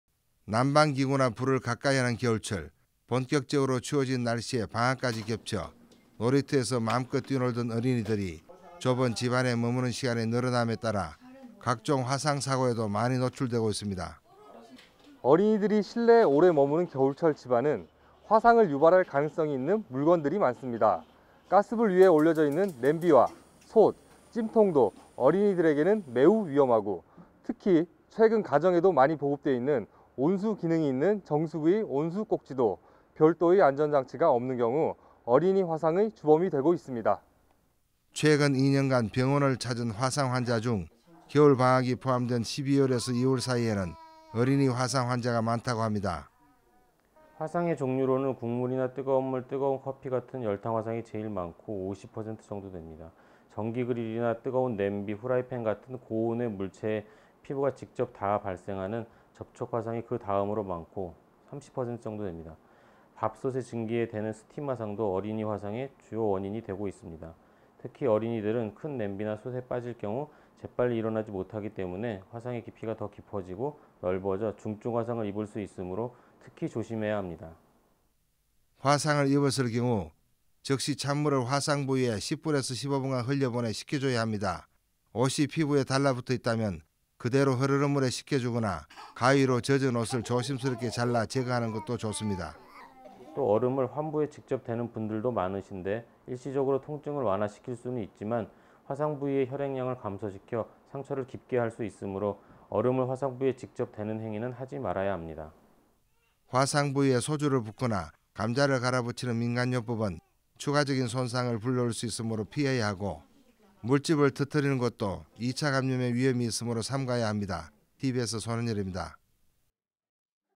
[tbsTV] 아차 사고, 평생 흉터! 어린이 화상사고
추운 날씨 탓에 어린이들이 집 안에서 노는 시간이 많은 겨울철!
어린이들이 집 안에서 당하는 화상사고가 많이 발생하고 있다는데요,
자칫 평생흉터로 남을 수도 있는 '어린이 화상사고'의 올바른 대처법에 대해 알아봅니다.